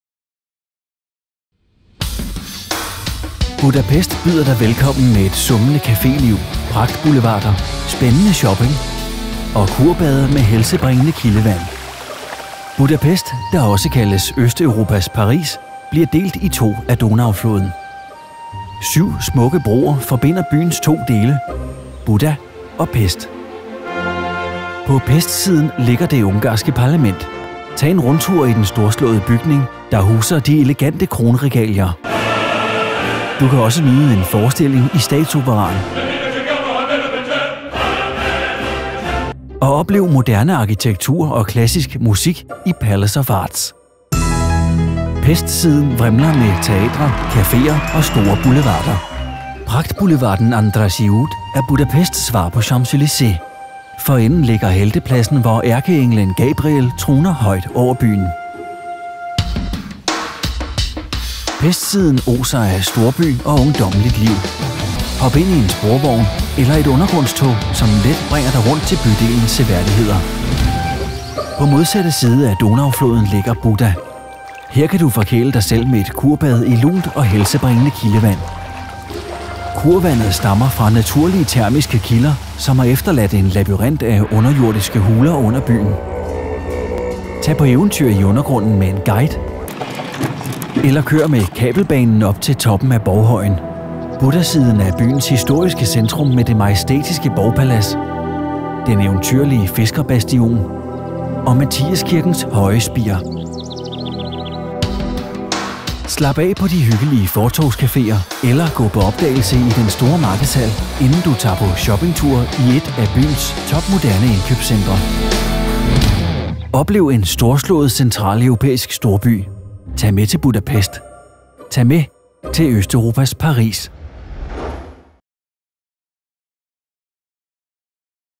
Budapest rejser - storbyferie i Budapest
Se video om storbyferie i Budapest og oplev Østeuropas Paris. Se flere rejsevideoer fra hele verden på og læs mere om storbyferie i Budapest på

Budapest byder dig velkommen med et summende caféliv, pragtboulevarder, spændende shopping og kurbade med helsebringende kildevand.

På rejser til Budapest oplever du Budapest svar på Champs Elysées Pragtboulevarden Andrássy út. For enden ligger Heltepladsen, hvor ærkeenglen Gabriel troner højt over byen. Du ser også Borghøjen med fiskerbastionen, borgpaladset og Matthiaskirken.